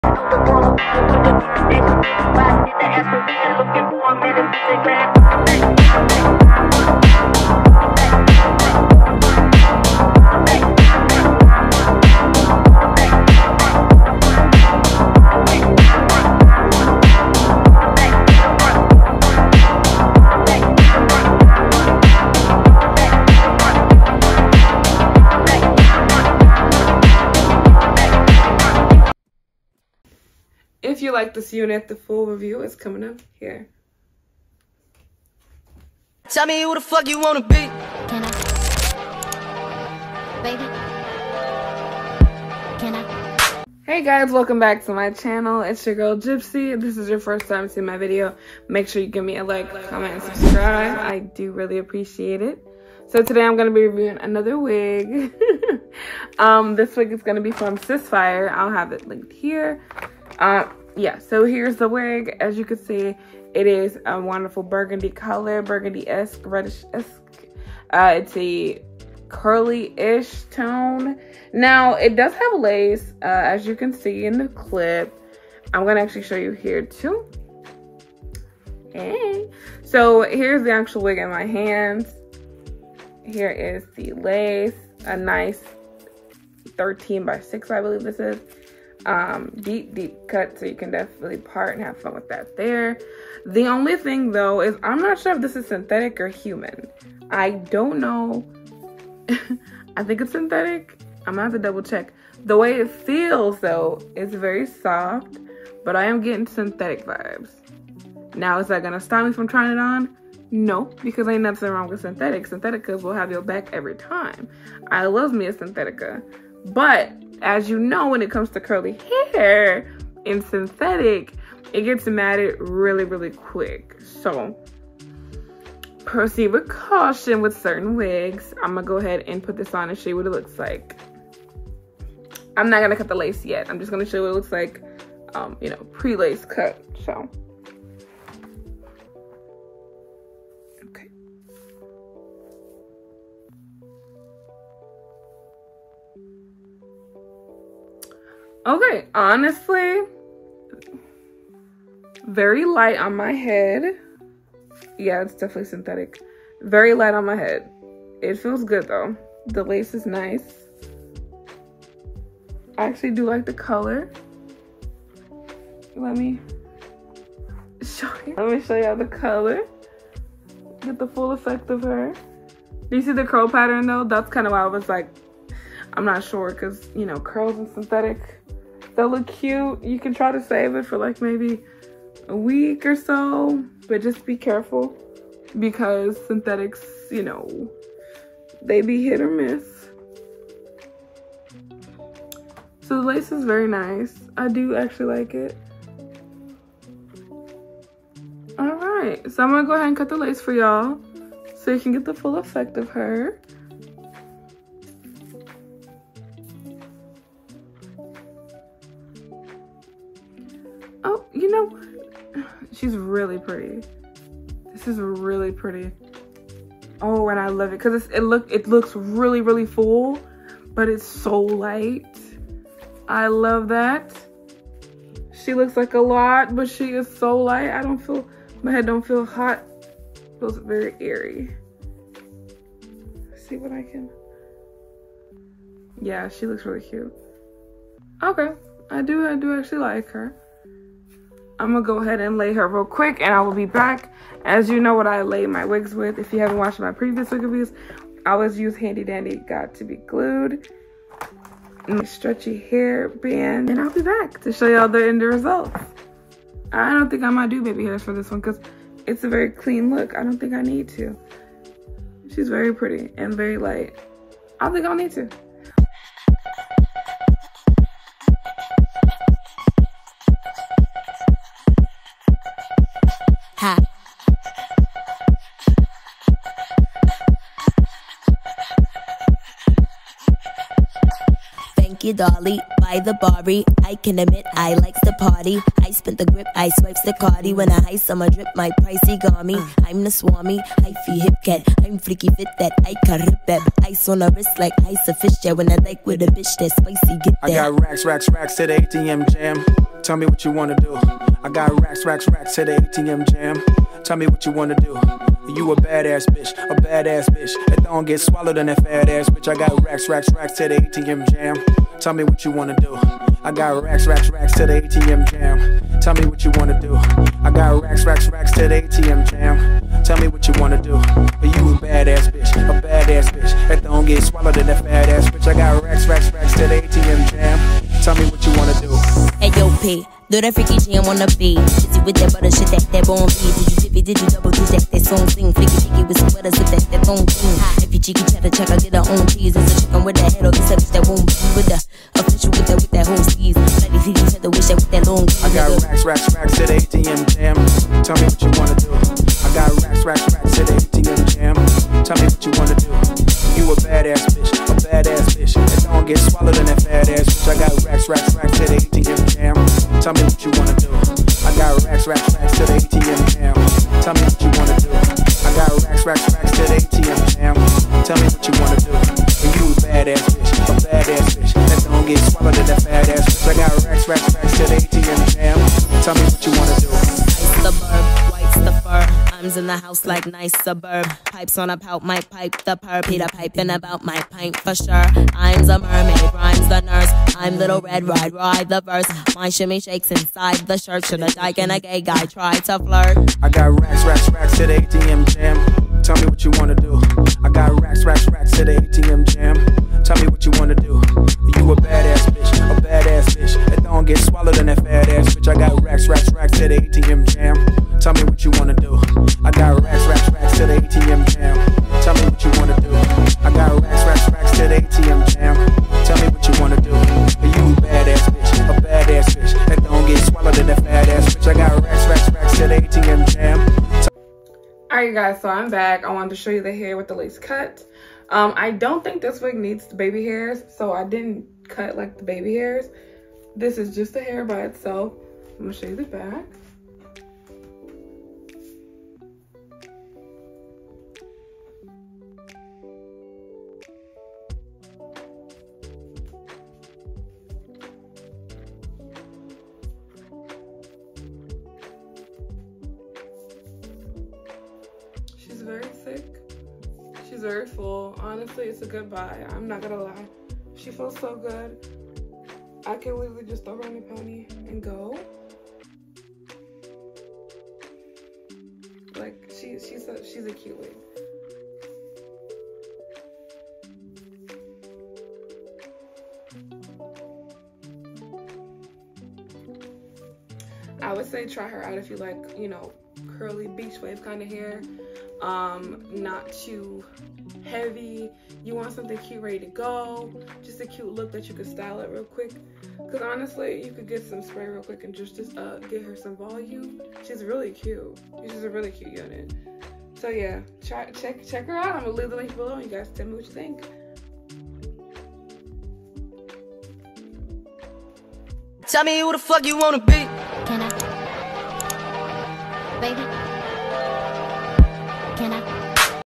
"💁‍♀️ Unveiling SISIFIRE Wig! Honest Review & Style Test | Wig Wonderland ✨
"🎁 Unboxing Amazon's Finest Wigs! Join me as we unveil the magic in each package, exploring style, quality, and the overall glam transformation. Get ready for a wig wonderland adventure! 💇‍♀️✨ AmazonWigUnboxing WigMagic"